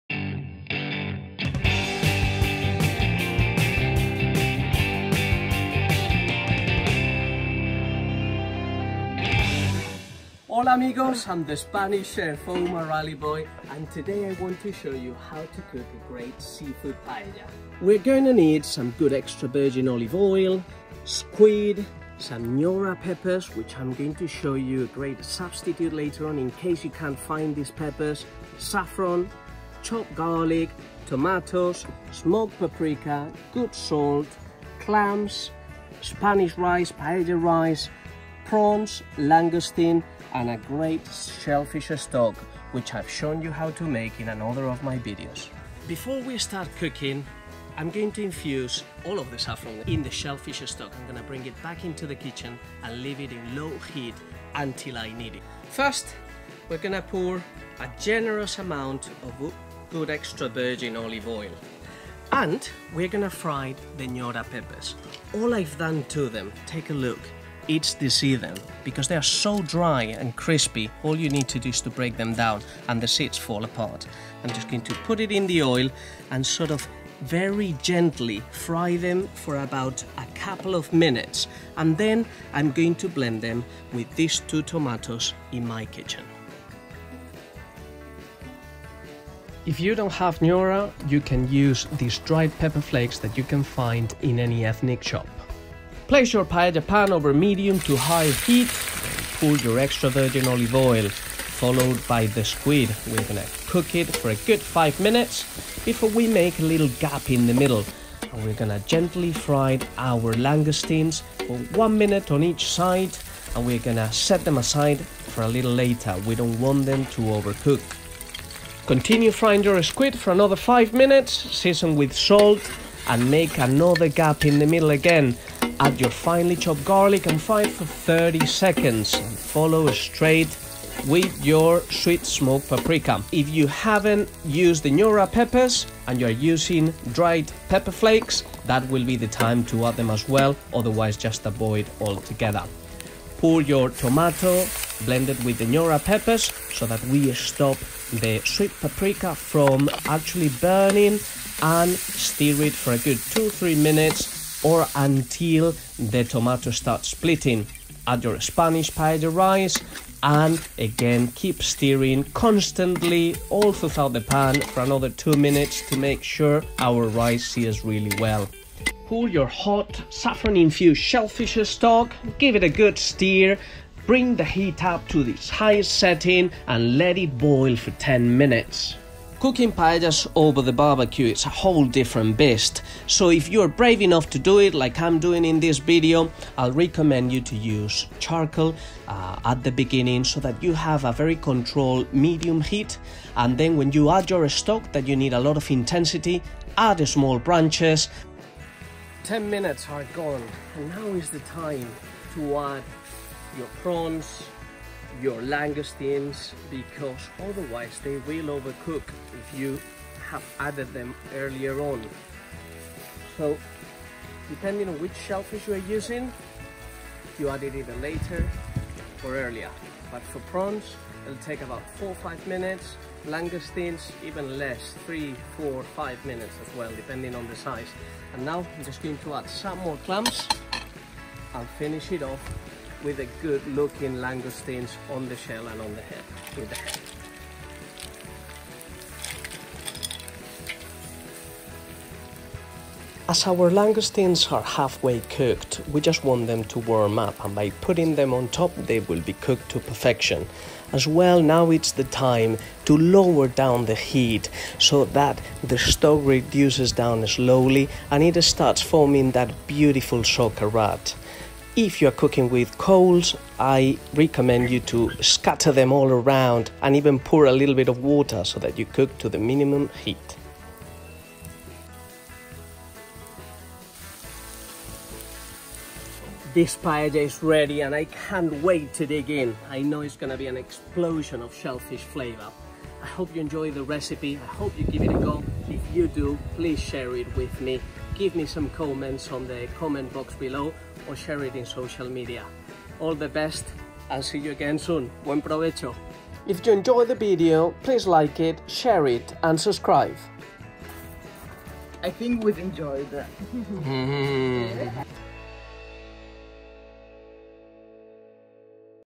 Seafood paella | Paella de marisco
This is a showstopper of langoustines, prawns, squid, and clams paella! One dish that will transport you straight into the Mediterranean coast of Spain, sunglasses on, holiday mood on, over 30 degrees, a light breeze to make it easier (I give you that), vino Blanco to hand and your family around the table. I mean, I am getting excited as I write this! This dish is a jewel of our gastronomy which should be truly celebrated. I share it with all of you with the hope that it brings you a lot of happiness around the table. Salud!

Alternatively, you can find this recipe and many others in my 2 bestselling books:

TAPAS REVOLUTION

SPANISH MADE SIMPLE

Gracias amigos,